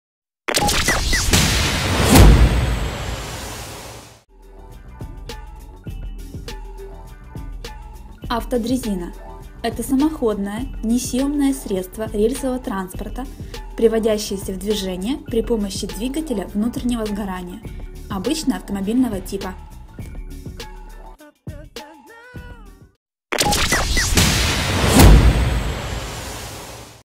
Автодрезина.Толковый Видеословарь русского языка
Что такое автодрезина. Дефиниция слова автодрезина. Автодрезина в Толковом Видеословаре русского языка.